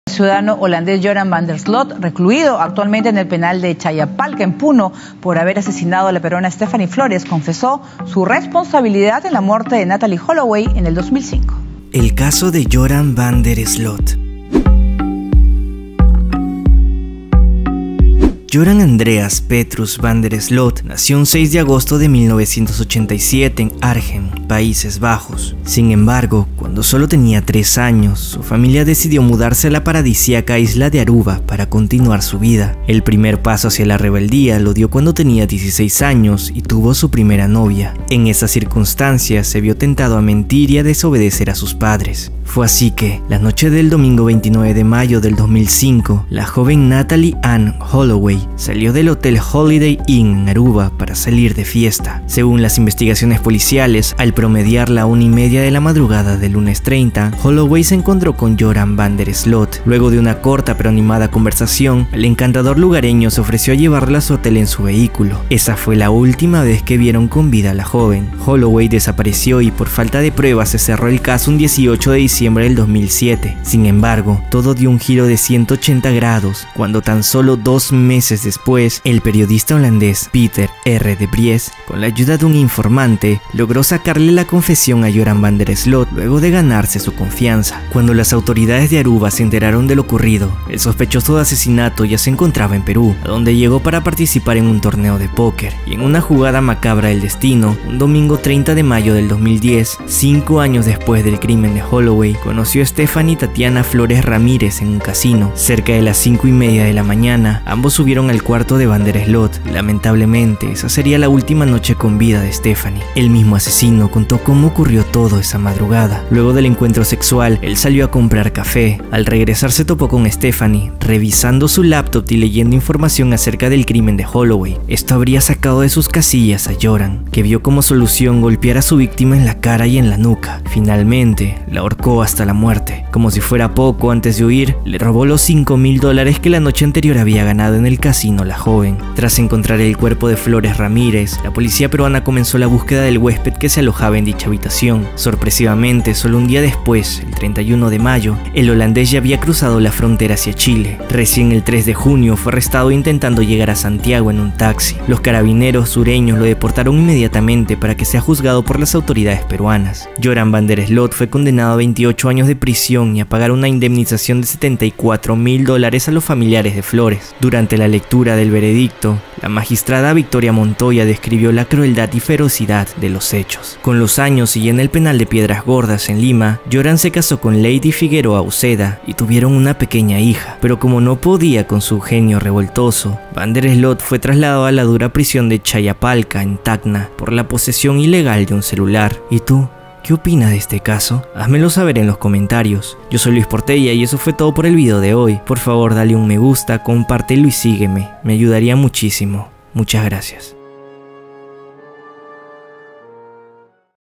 El caso de Joran Van Der Sloot | Luisito Portella
Hoy hablaremos del caso del asesino Joran Van Der Sloot, cuya historia conmocionó a todo el Perú hace ya varios años.